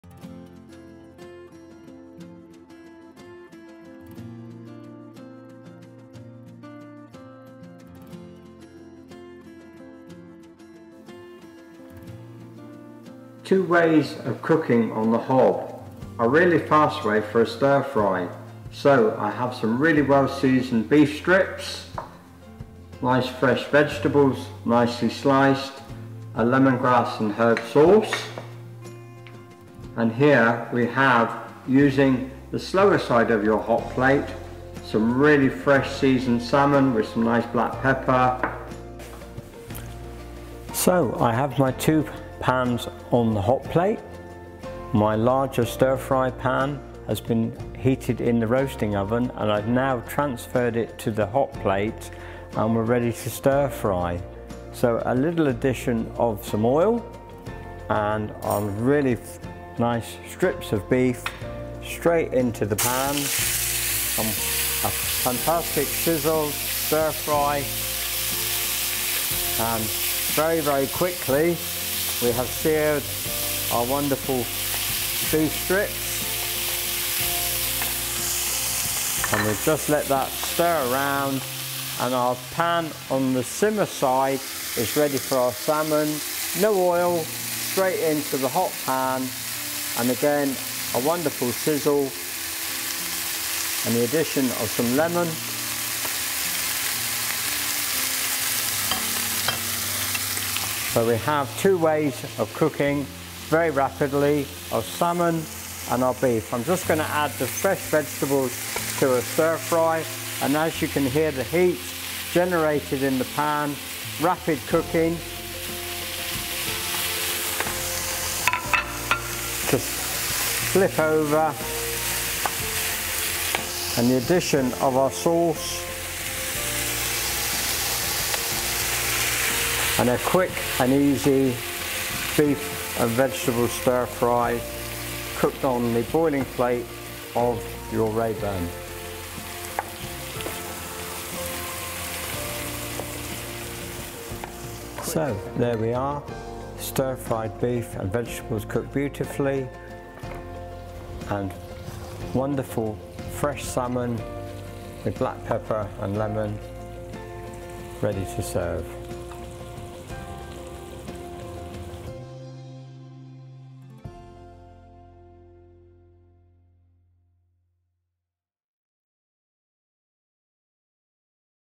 Rayburn cooking with David Pengelly and Rangemoors. The sizzling Rayburn Stir Fry.
David Pengelly shows how to cook a stir fry on the hottest plate of the Rayburn.